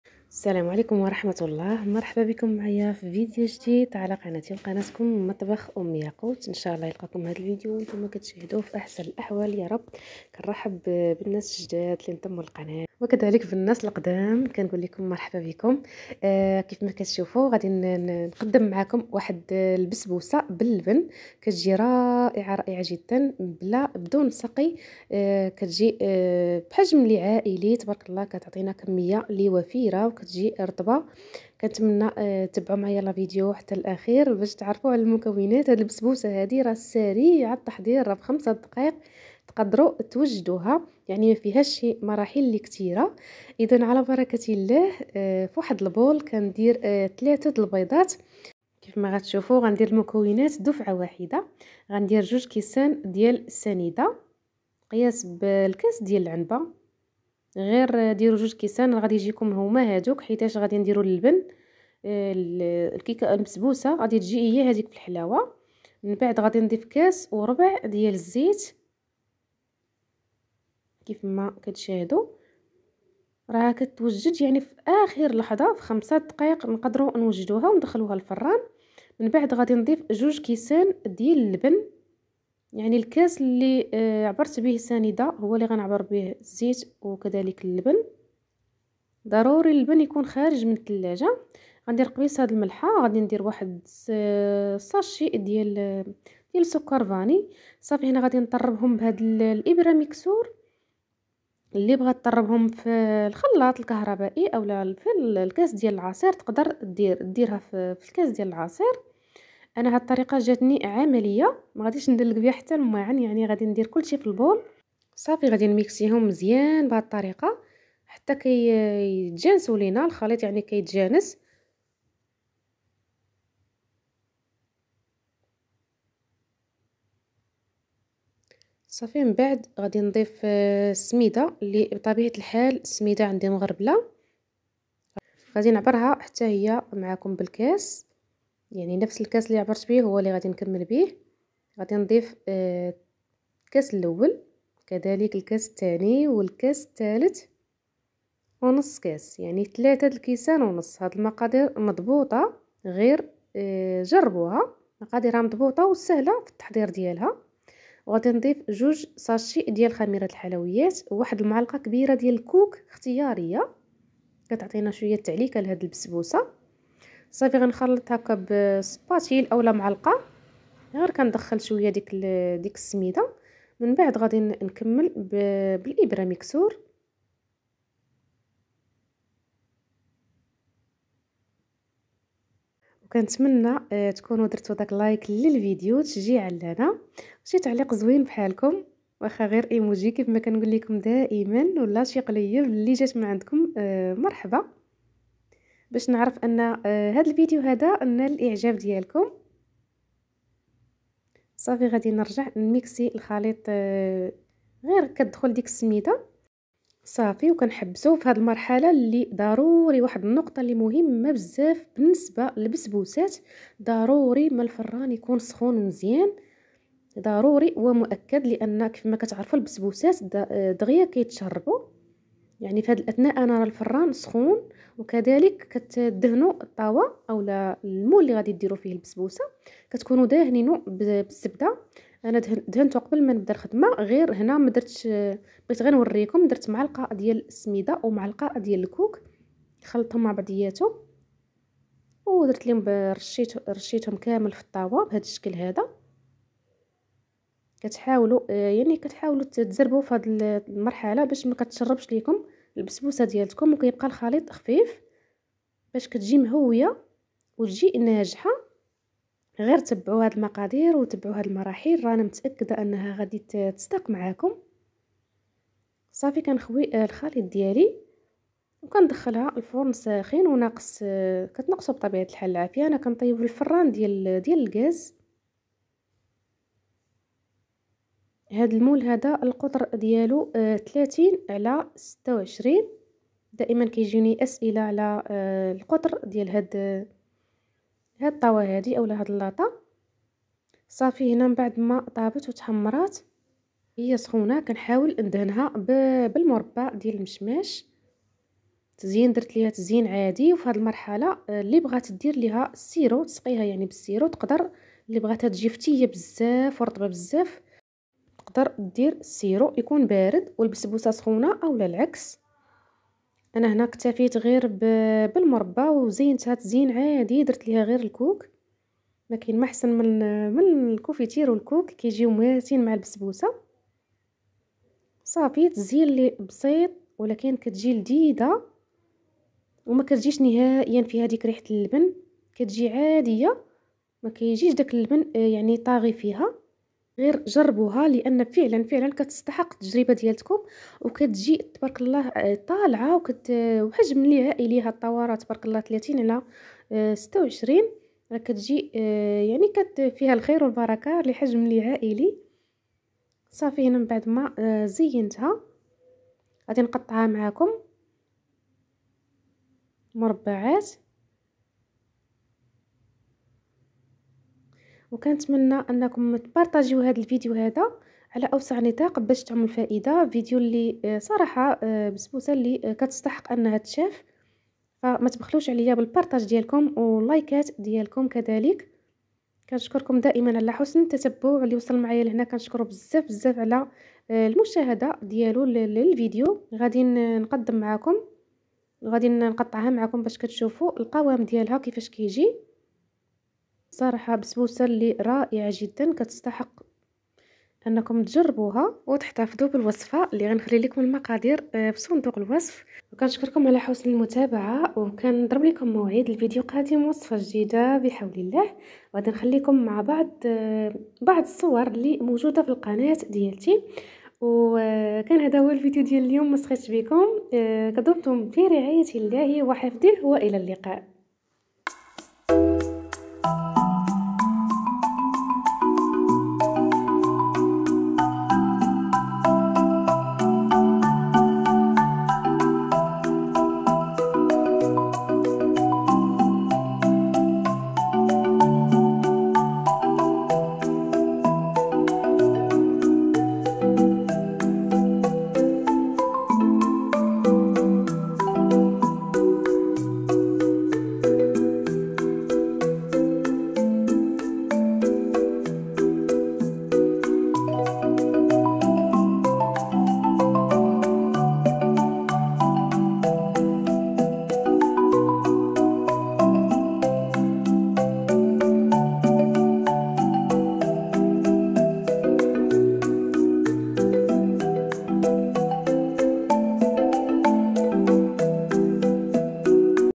بسبوسة الخمس دقائق أروع ماممكن تجربي كتوجد فآخر لحضة
المكونات 👇👇✅✅

3بيضات
قبصة ملح
2 كأس من السكر
كأس وربع من الزيت
ساشي فاني
2كأس من اللبن
3كؤوس ونصف من السميد الخاص بالحرشة والبغرير
2ساشي من فئة 7جرام من خميرة الحلويات